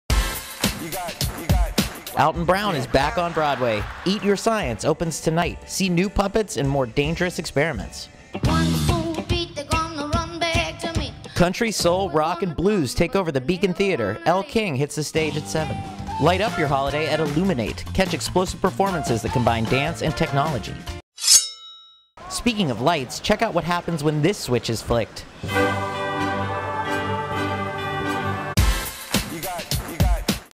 LIGHT UP - NOV 22
On Broadway, Alton Brown is back with a new show, Eat Your Science. Plus, light up your holiday at Illuminate or see Elle King in concert.